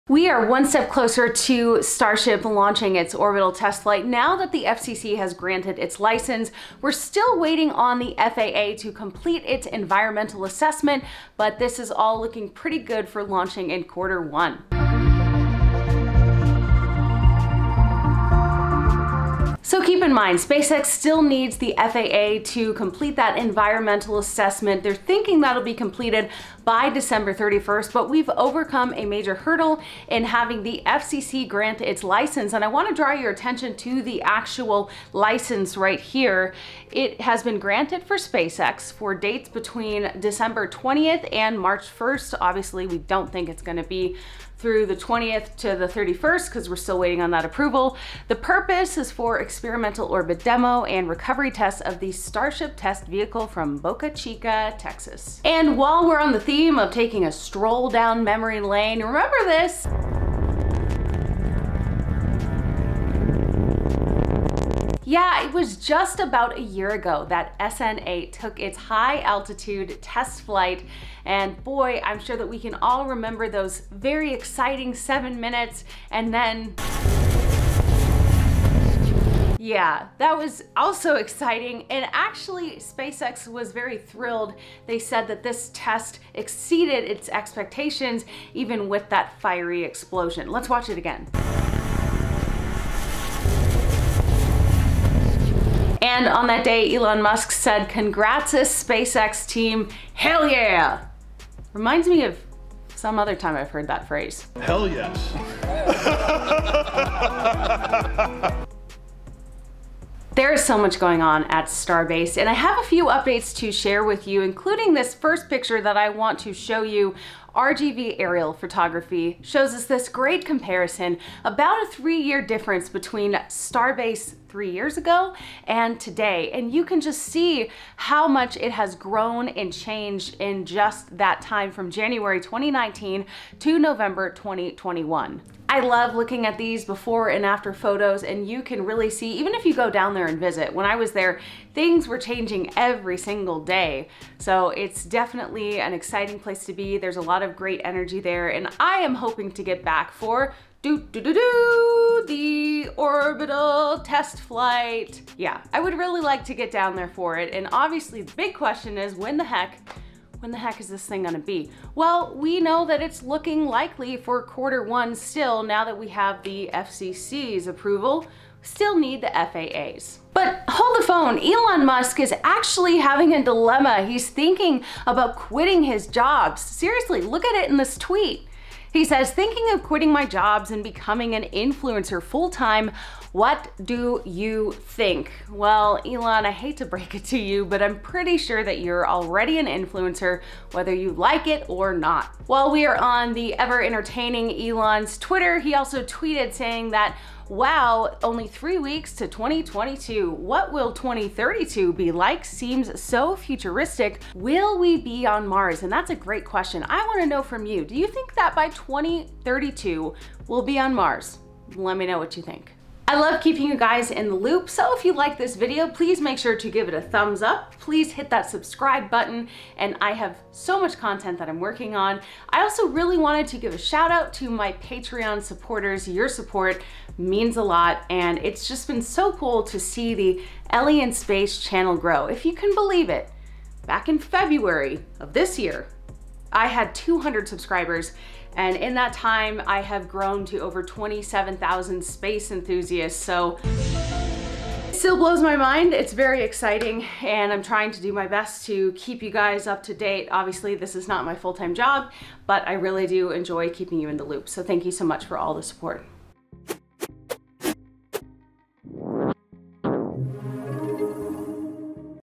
Starship one step closer to orbital test flight in Q1
Elon Musk's SpaceX gets the green light from the FCC for orbital launch of Starship early next year - but it will still need to pass FAA environmental review before the rocket can leave the ground.
SpaceX needs the FAA environmental assessment completed to be clear for Starship's first orbital test flight, but this FCC license shows the company is still looking at launching in Q1.

Howdy!
I am a news reporter in beautiful Salt Lake City with a passion for all things space.

Intro and Outro credit: YouTube: Lloydwing